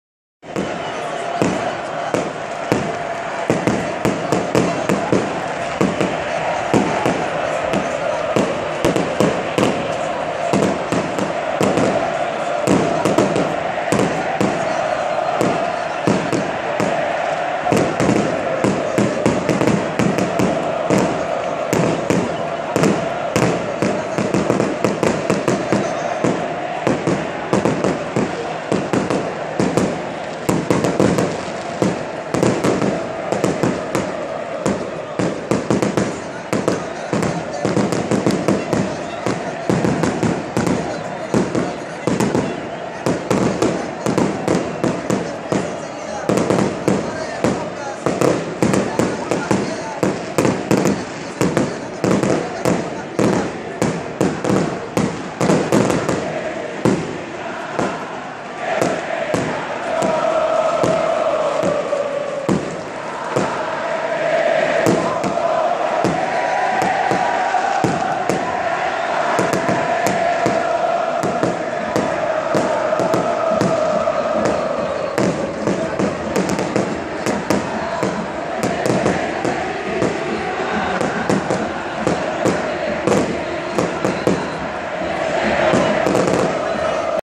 Si es noche de Copa es noche de Olimpia!
Una nueva historia, en busca del único título internacional que le falta, comenzó Olimpia. El primer paso está dado: 2-0 ante Huachipato de local. Ahora, habrá que asegurar ese ventaja en Chile para avanzar a la siguiente fase de la Copa Sudamericana.

En una nueva noche de Copa, de las tantas que ya tiene el Decano, así fueron recibidos por la afición franjeada los dirigidos por Chiqui Arce.